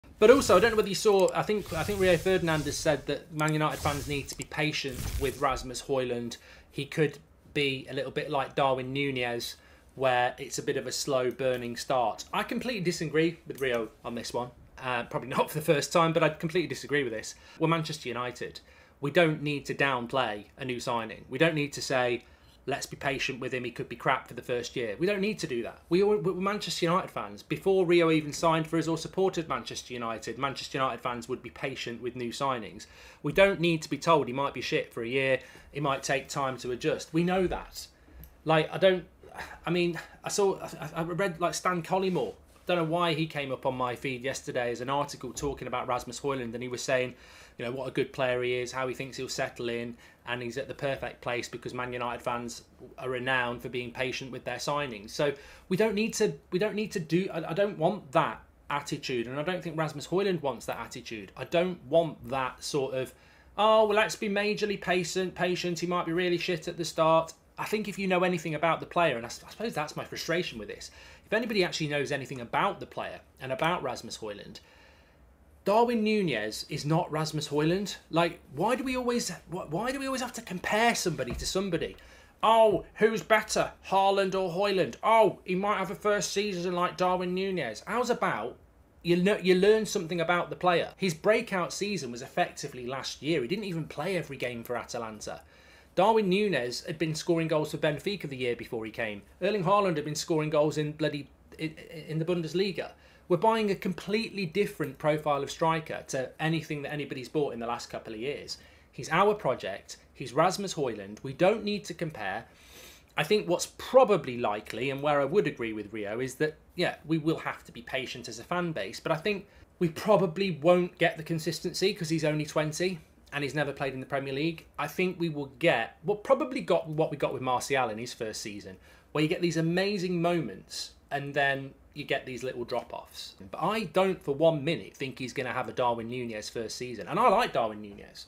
Mark Goldbridge Disagrees With Rio Ferdinand Over Højlund's First Year
Mark Goldbridge completely disagree with Rio and his comments about Hojlund's first year at Manchester United.